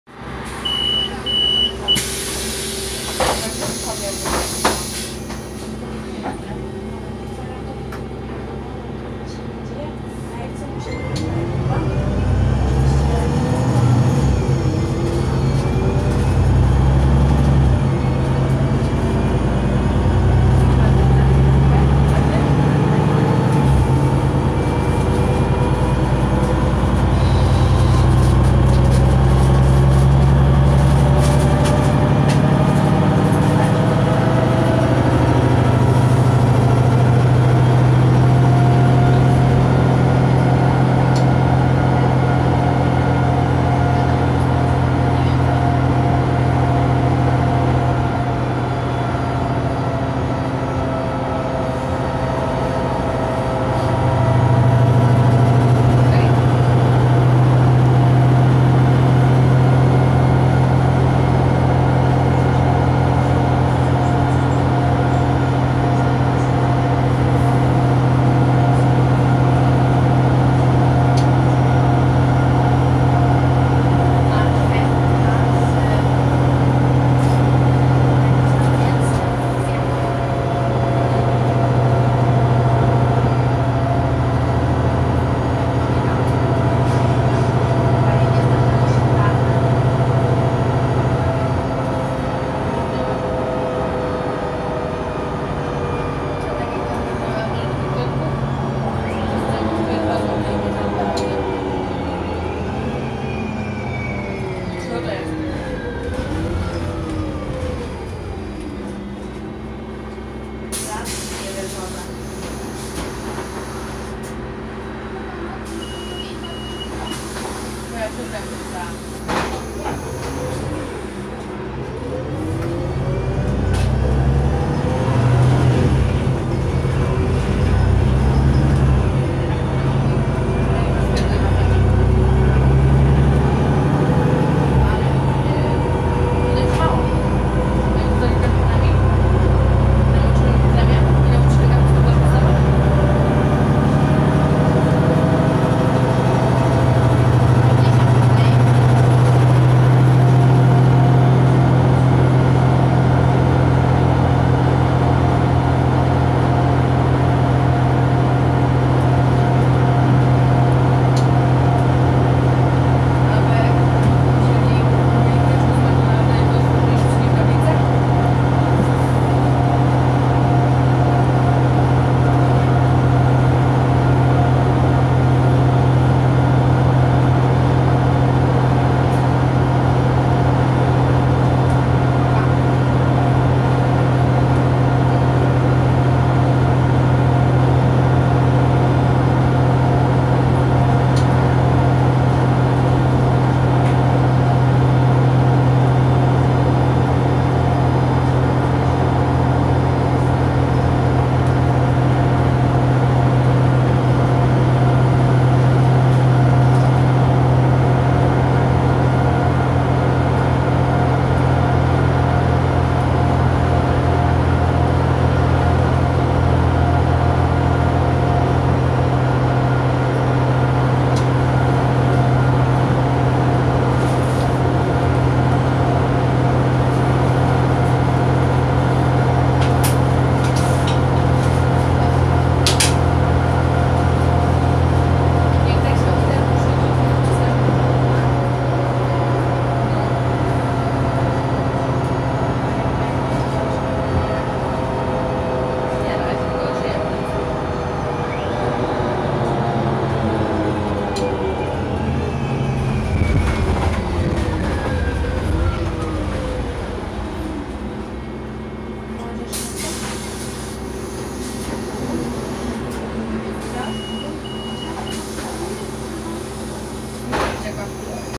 [DLA Wrocław] Solaris Urbino 10.5 IV #2020 /948 kier. Miękinia (Pętla)\
Jazda po fragmencie drogi krajowej numer 94 w autobusie linii 948, który na moje spostrzeżenie jeździ jak PKS, zatrzymuje sie w wioskach o dużych odległościach między sobą i małej ilości kursów oraz jeżdżą po jakiś ważniejszych drogach (jak w tym przypadku - Krajówka)

Pozdrowienia dla miłego kierowcy ;)

Dane techniczne:
Silnik: Cummins ISB6.7E6C
Skrzynia biegów: Voith D854.6
Rok produkcji: 2019

Przystanki:
1. Żar (Koniec Strefy Miejskiej Wrocławia)
2. Krępice
3. Wróblowice